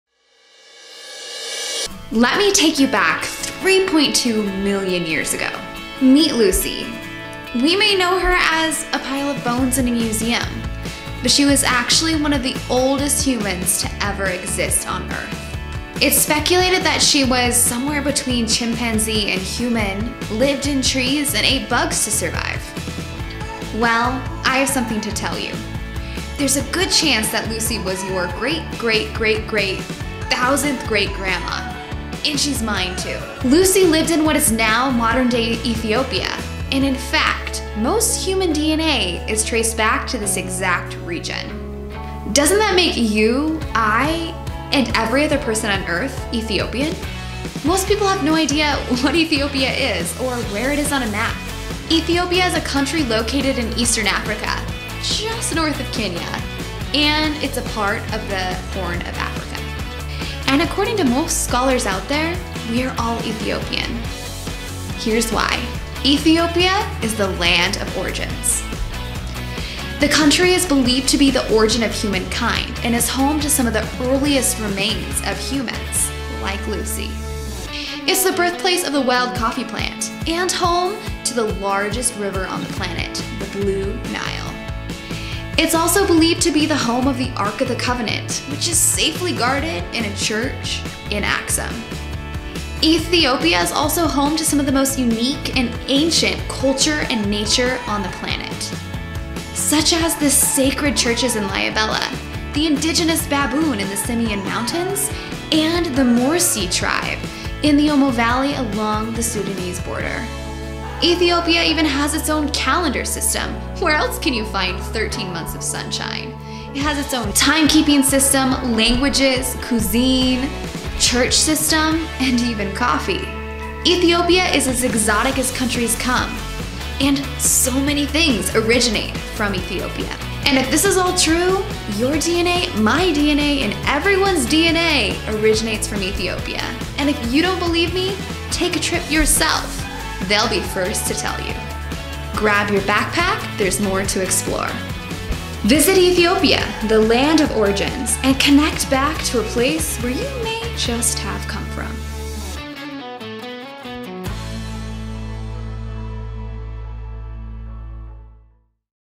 We Are All Ethiopian
Most people have no idea what Ethiopia is or where it is on a map. Ethiopia is a country located in Africa and is known as the Land of Origins, including the Origin of Humankind...

We are all Ethiopian, here's why!

Visit Ethiopia, the land of origins, and connect back to a place where you may just have come from.